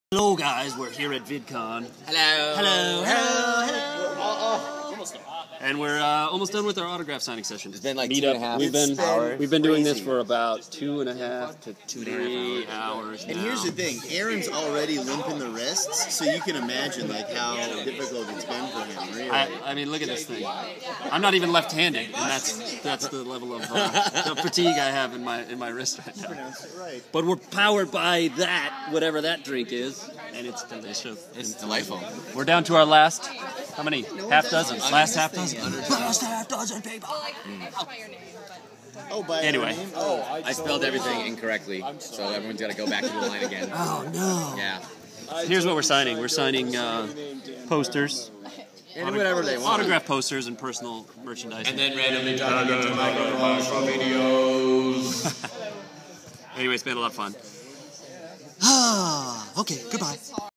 Annoying Orange Meetup - VidCon2012
Hanging out with the Annoying Orange cast and fans at VidCon!